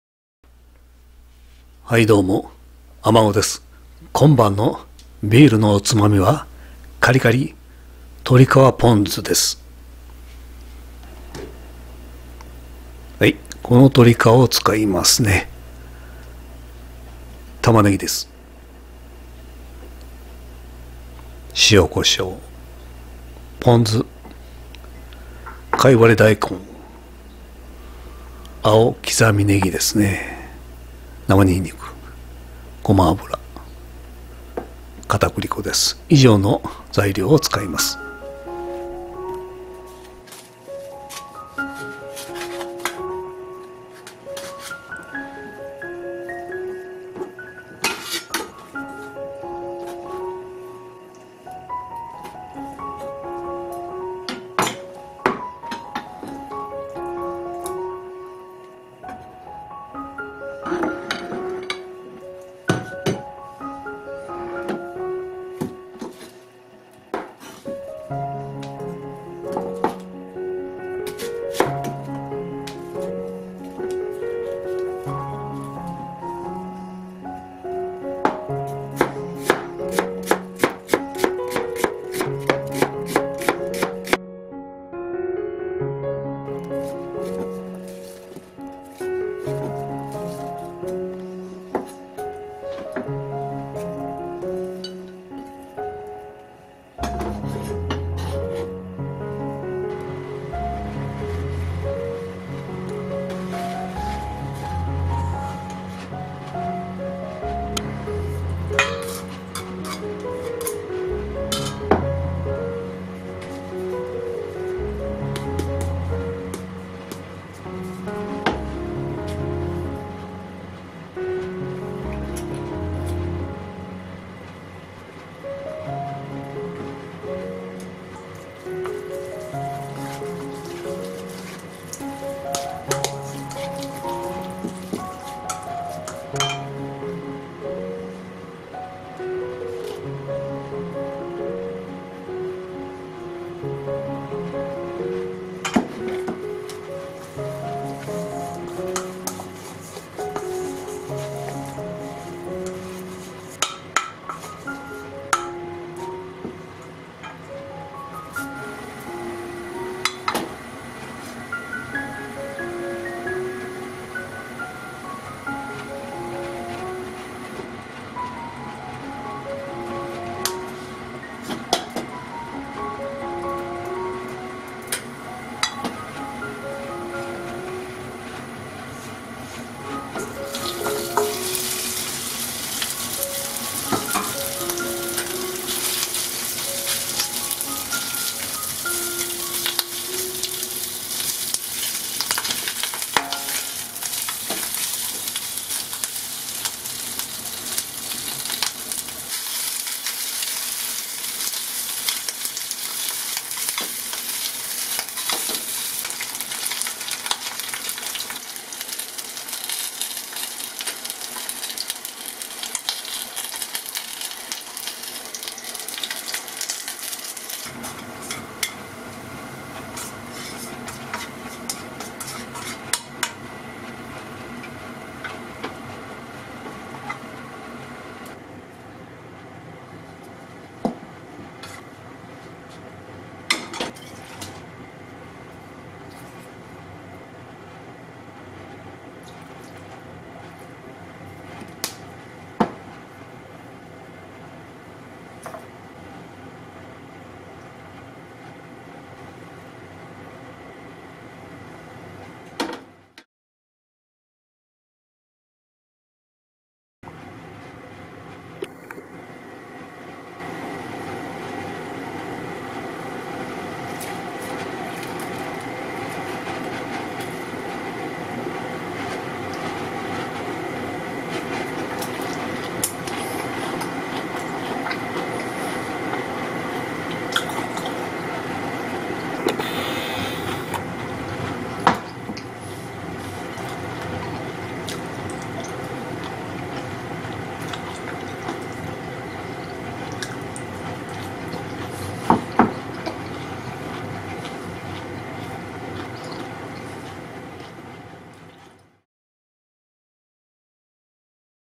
ビールがすすむ！居酒屋メニューのカリカリ鶏皮ポン酢
鶏皮を少し切る。玉ねぎも切る。
鶏皮に塩コショウ又はブラックペッパーなどを混ぜる
好みで "生にんにく"も入れます。小麦粉を混ぜます。

フライパンに ごま油を引き鶏皮を強火で炒める
鶏皮がほぼ炒め上がったら取り出し 玉ねぎ少し炒めます。
ポン酢をかけて青ネギを振りかけていただきます。
付け合せにピリッ!と辛い カイワレ大根やキウイを添えても良いね！

鶏皮は高カロリーですが
摂取量が直接血中総コレステロールに反映されるわけではないようです。
が・・沢山食べると やはりね！

材料：鶏皮、タマネギ、片栗粉、塩コショウ、きざみ青ネギ、
ポン酢、ごま油、カイワレ大根など